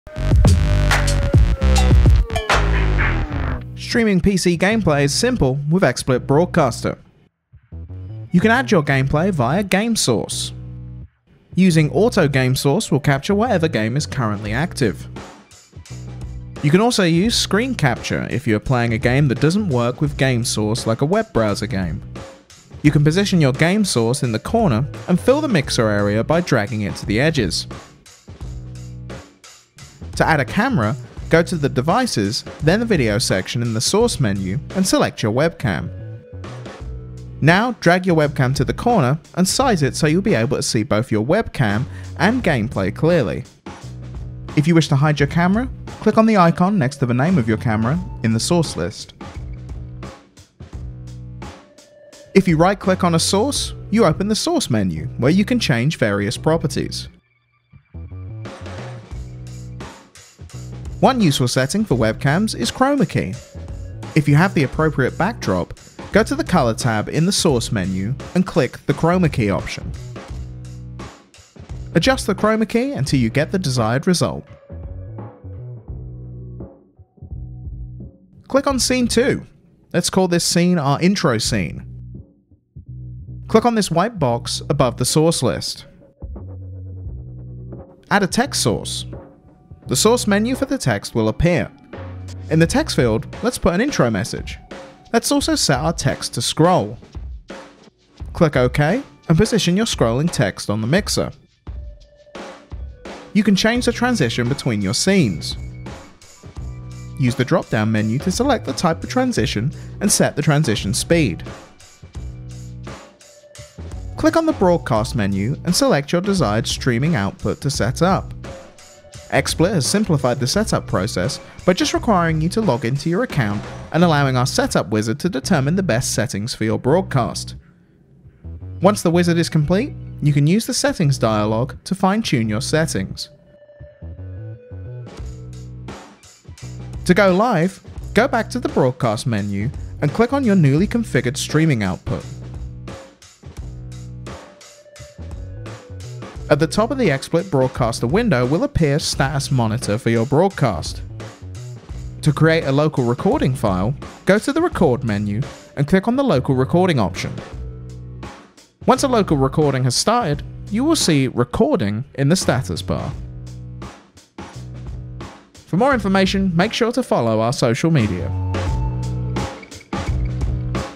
How to Stream and Record PC Games with XSplit Broadcaster
Learn how to stream or record PC Games with XSplit Broadcaster.